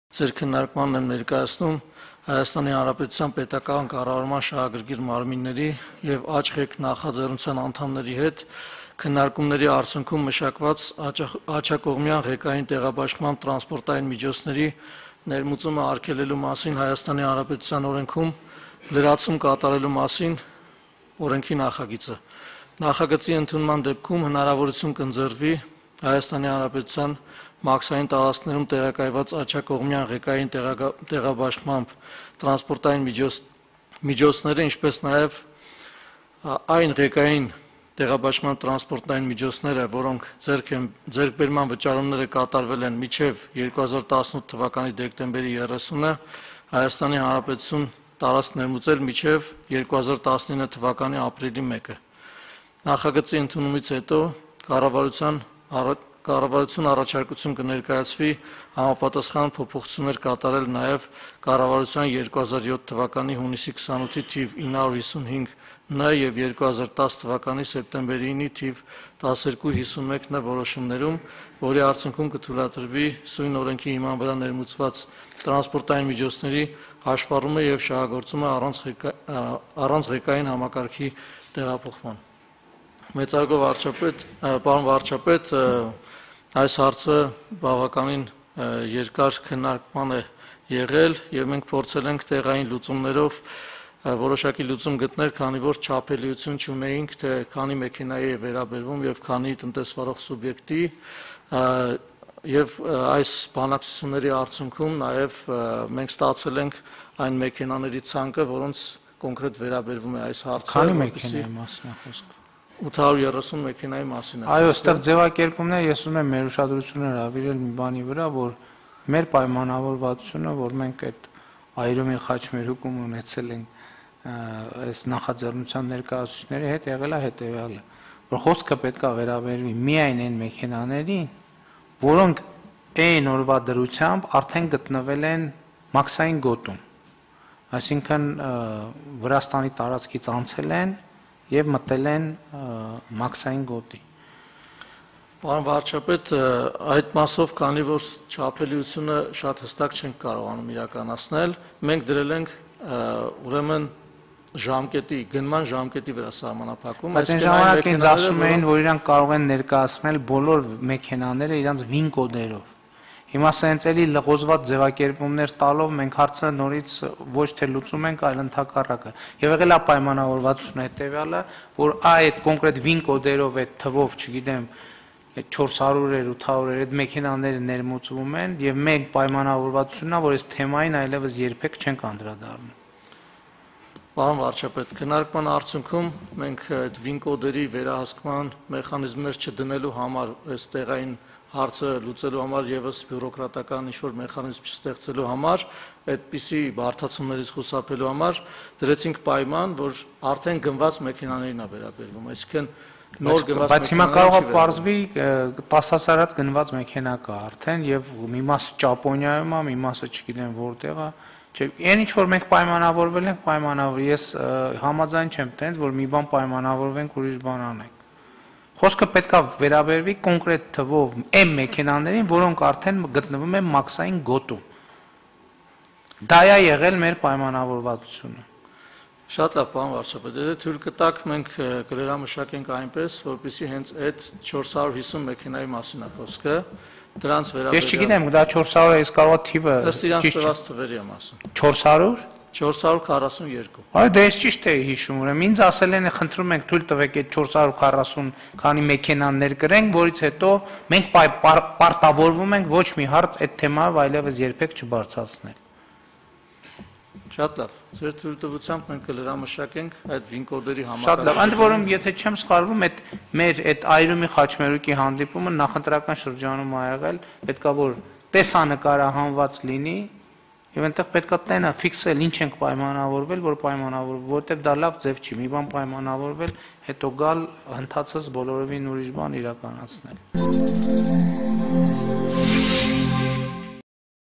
Աջ ղեկով մեքենաները մինչև ապրիլի 1-ը ներմուծել թույլատրելու հարցը հանվեց կառավարության օրակարգից
Վարչապետի առաջարկով աջ ղեկով մեքենաները մինչև ապրիլի 1-ը ներմուծել թույլատրելու հարցը հանվեց կառավարության օրակարգից